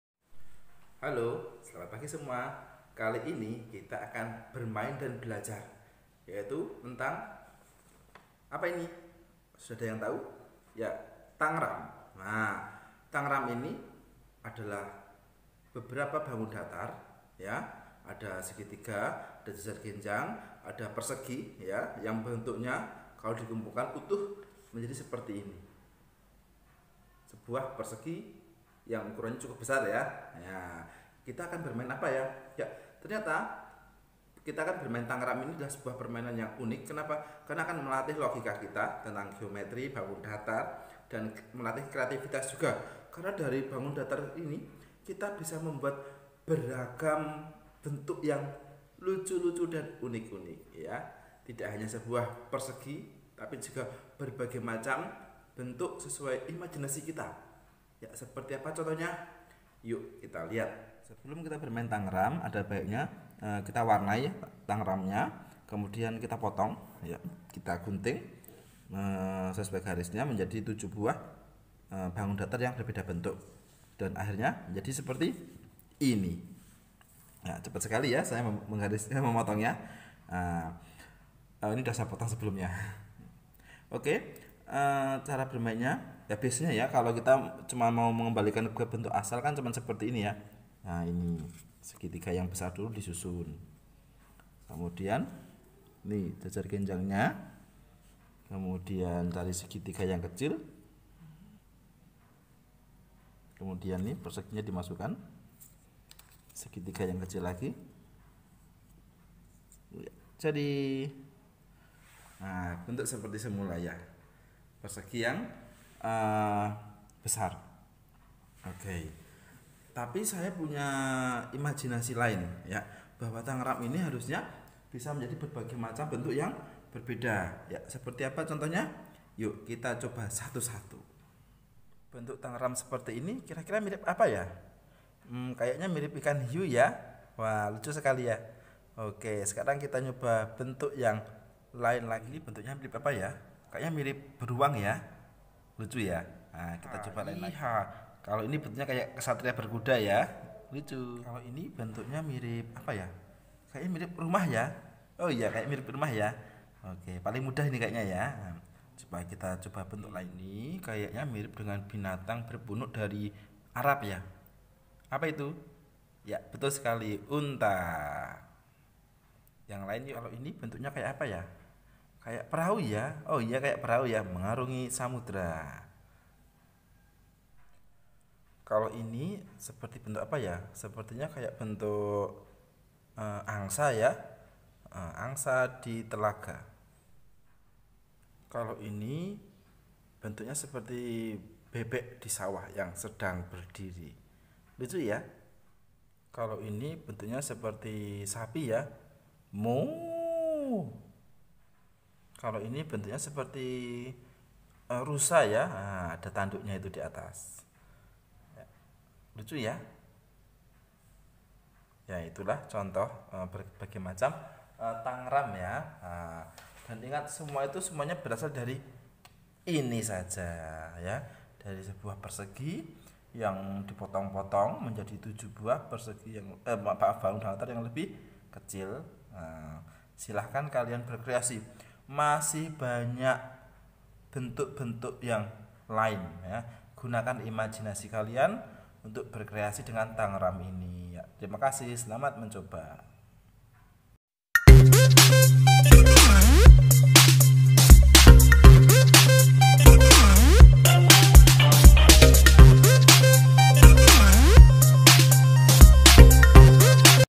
Yuk Bermain Dan Belajar Dengan Tangram
Tangram merupakan permainan edukasi yang terdiri dari 7 keping bentuk bangun datar yang saat ini banyak dipelajari. Tantangan dalam permainan tangram adalah menyusun kepingan-kepingan tersebut menjadi sebuah bentuk lain yang dicontohkan. Biasanya menjadi bentuk orang, rumah, burung, kucing atau bentuk-bentuk lain yang membuat setiap manusia yang melakukan permainan merasa senang.